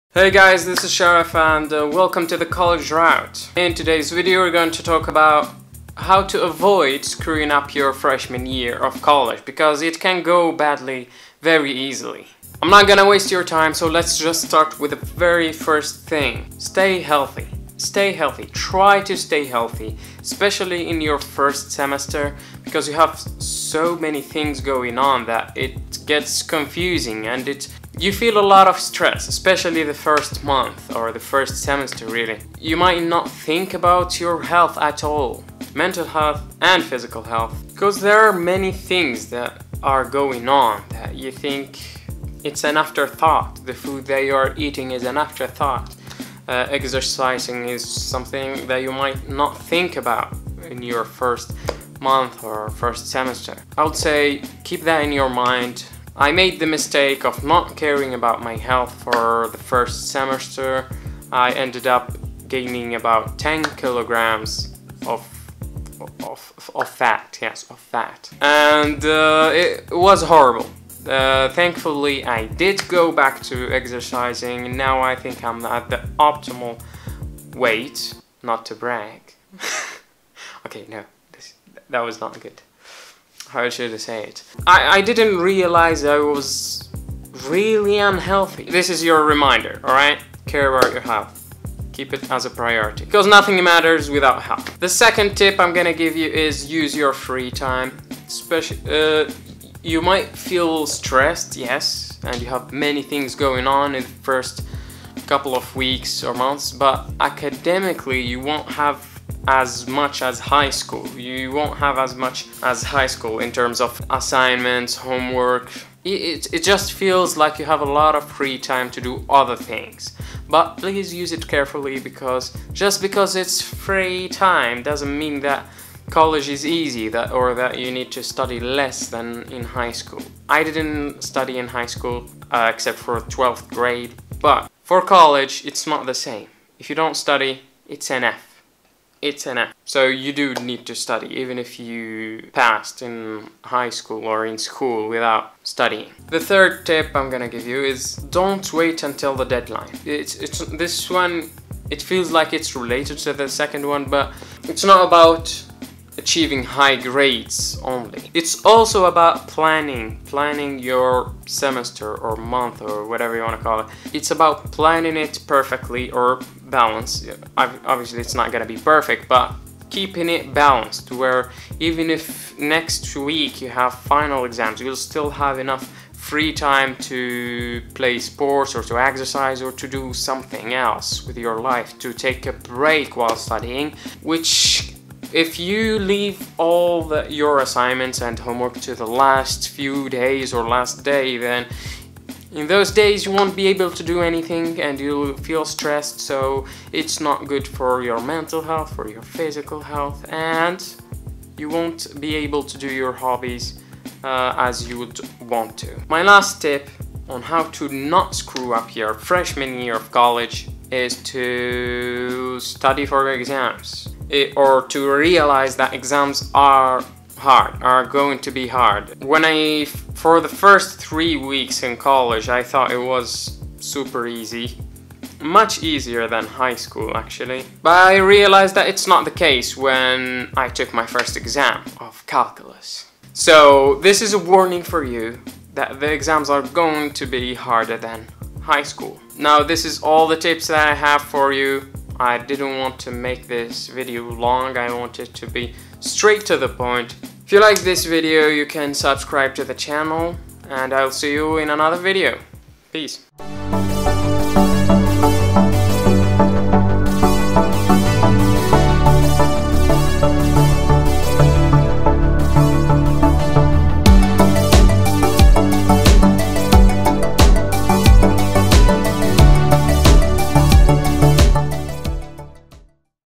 How to NOT screw up your freshman year in college
In this video I would like to share with you tips that I wish I knew before my college freshman year. Let’s not waste time and let’s get straight into it.